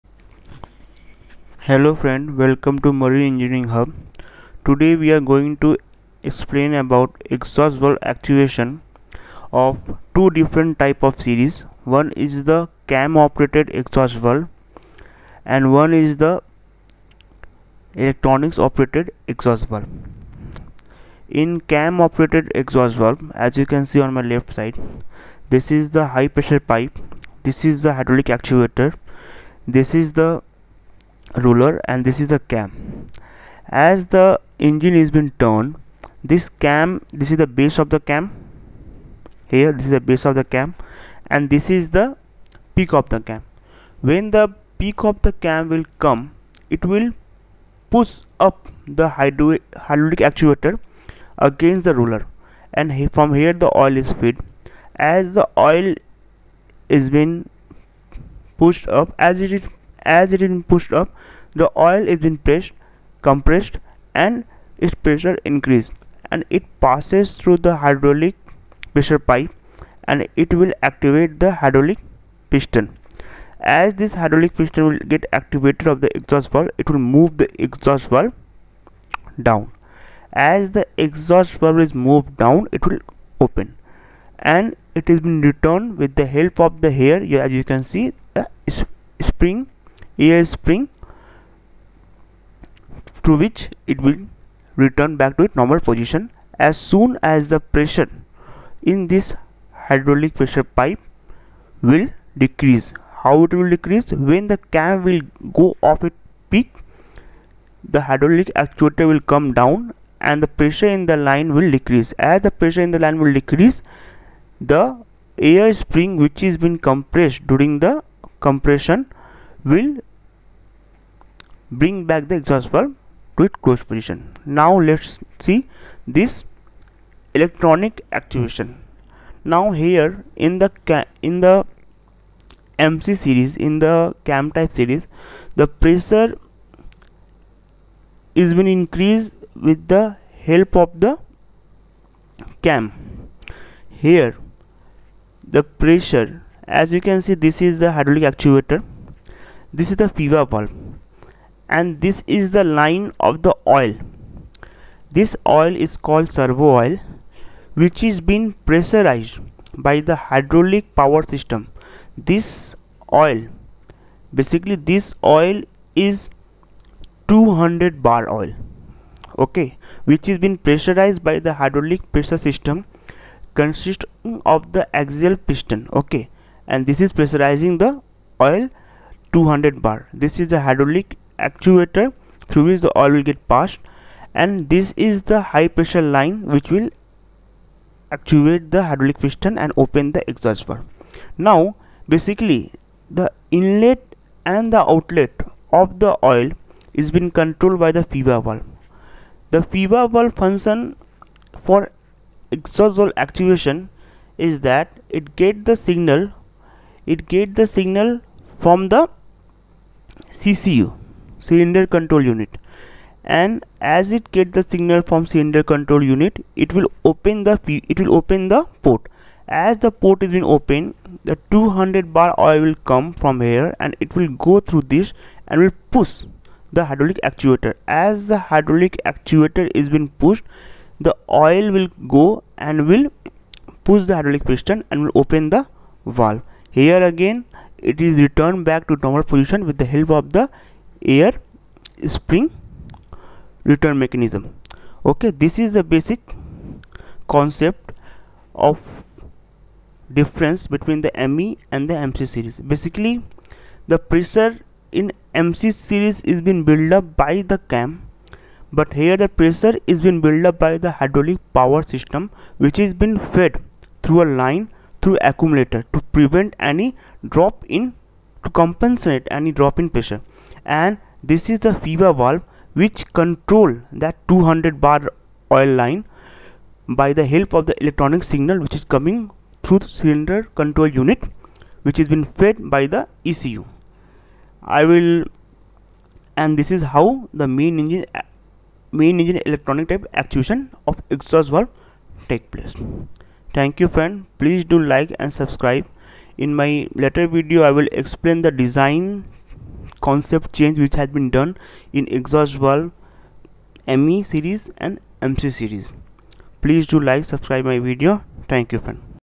difference between MC and ME engine of Exhaust Valve Actuation system ?
how Exhaust valve  actuation system work in ME series of MAN engine? Exhaust valve  actuation arrangement in MC series of MAN engine? How FIVA valve is controlling Exhaust valve actuation in ME engine?
Why  Exhaust cam arrangement is removed in ME engine? How  Exhaust valve  actuation of MC and ME differ in design? why Damper are provided in Exhaust valve?

In this video i have explained all these thing

If you want to know more about  you can click on below link:

(difference between MC and ME engine in designing of fuel oil system arrangement|ME Engine|)